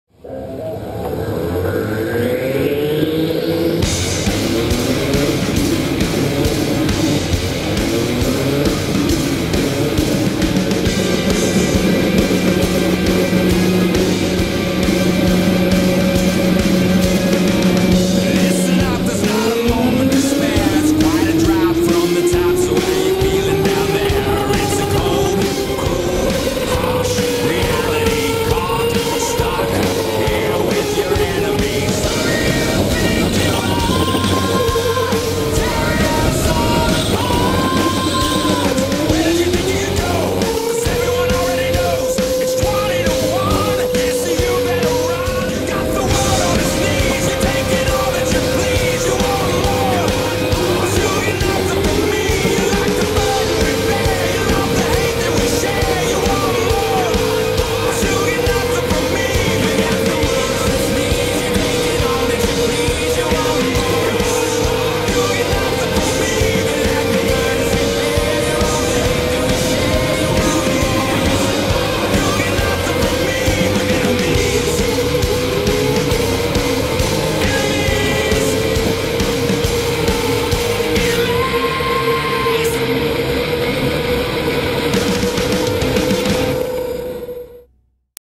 MB2 Raceway Edit-Clovis
Racing at MB2 indoor kart racing. Filmed on GoPro Hero2.